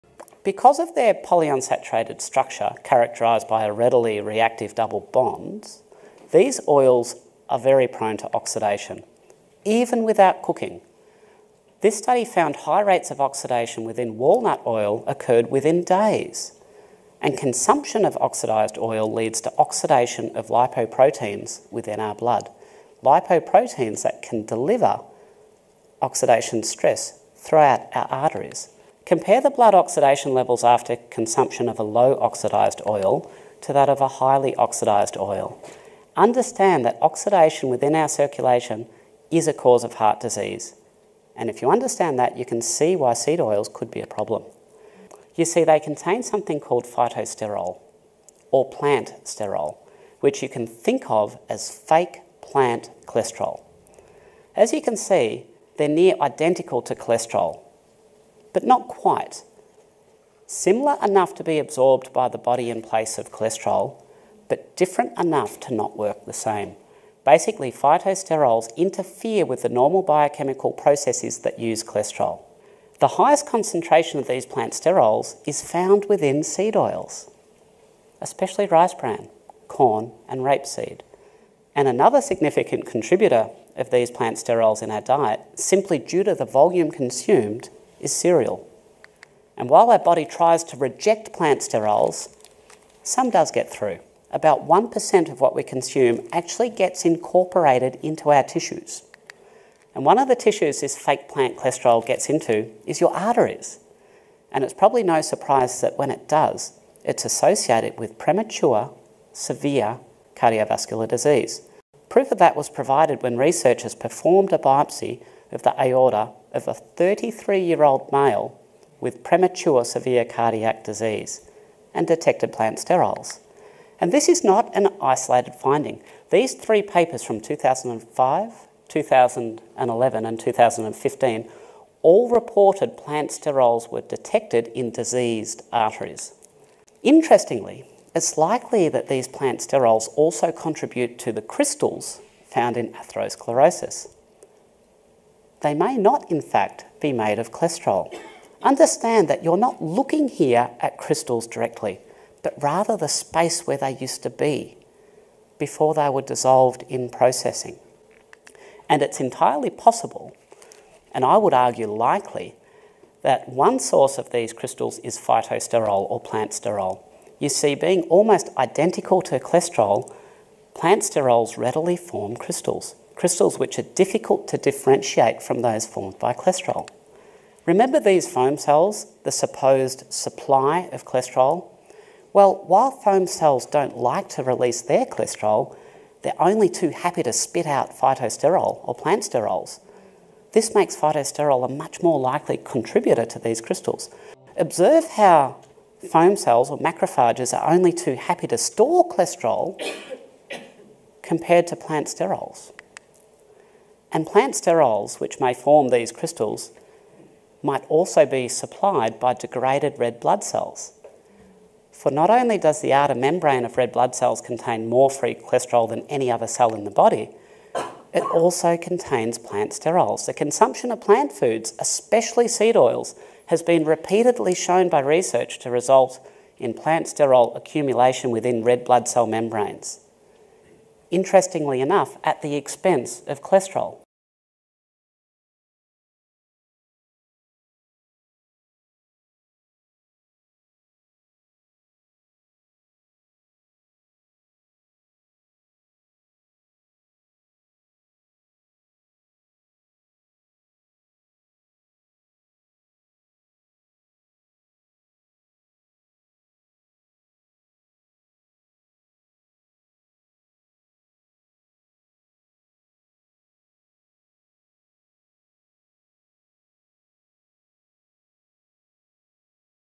PAUL MASON | TOP HIT 21: Plant Sterols: …LIKELY CAUSE OF atherosclerosis
presents episode 2133 | DR PAUL MASON

It has the links to original interview